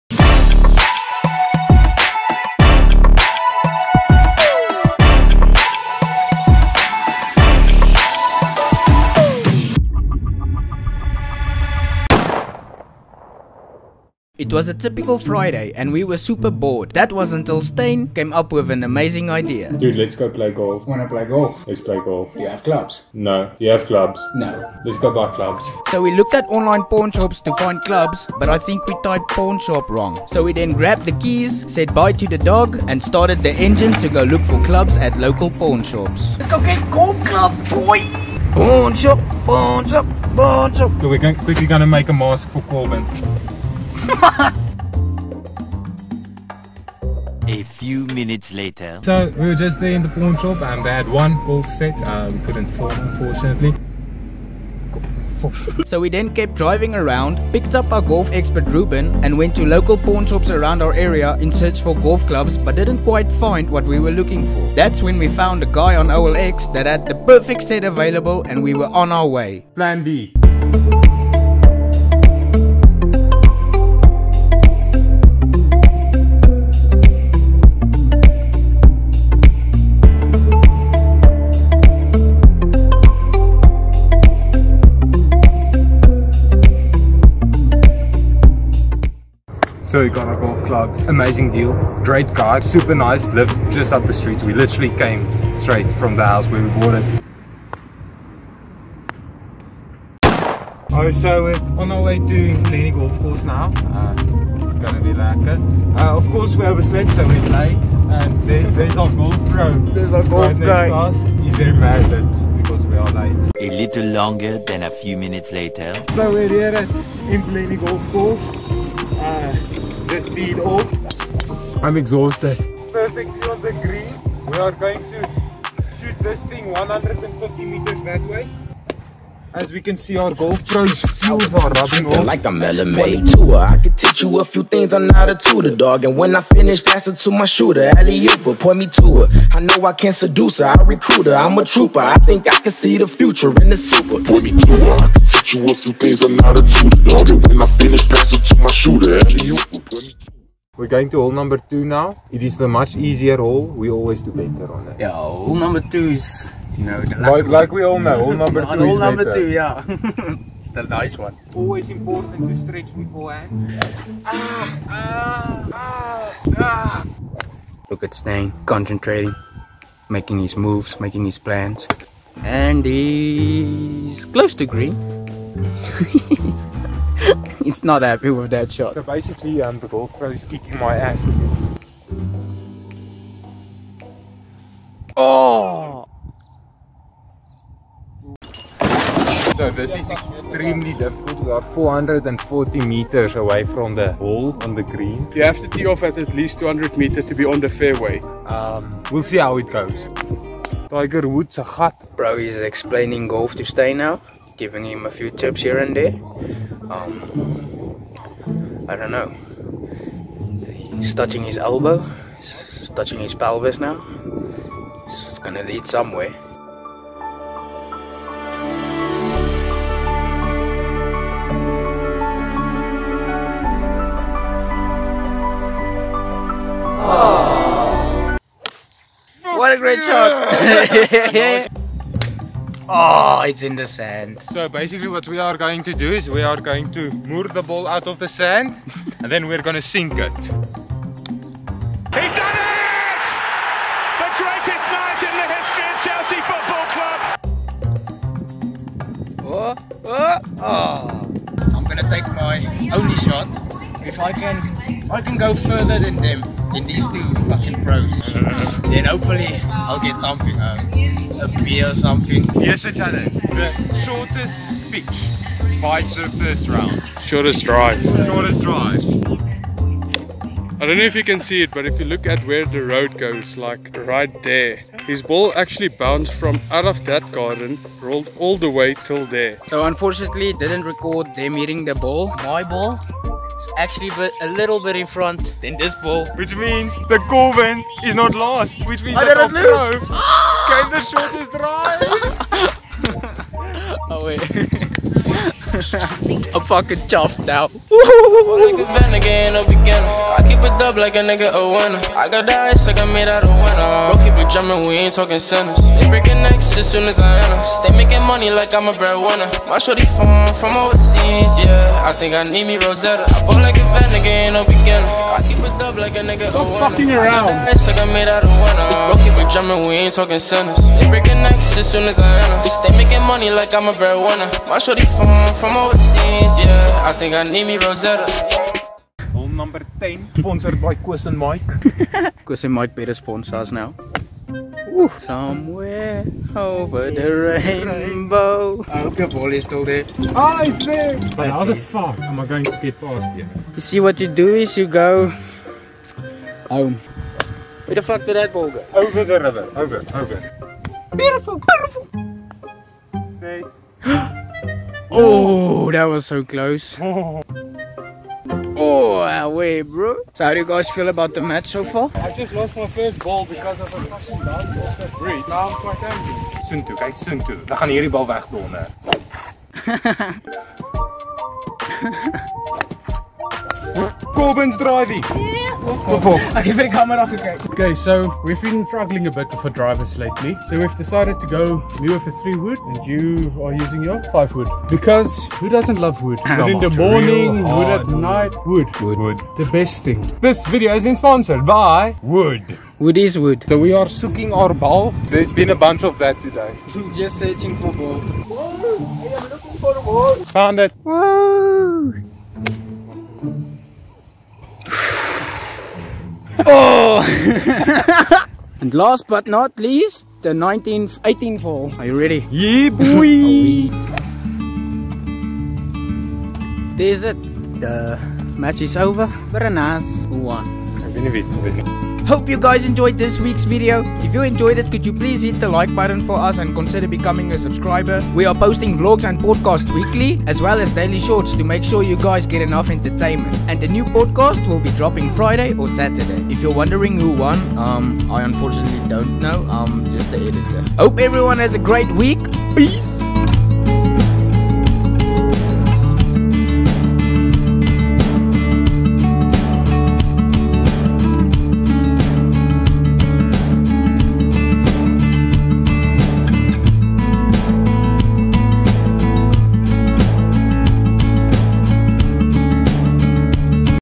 We Challenged a Golf PRO and Won!!
WE ARE US gets bored and decides to buy a set of golf clubs and go play golf against a pro! WATCH TO FIND OUT WHO WINS!!
☑ Viewed
☑ Liked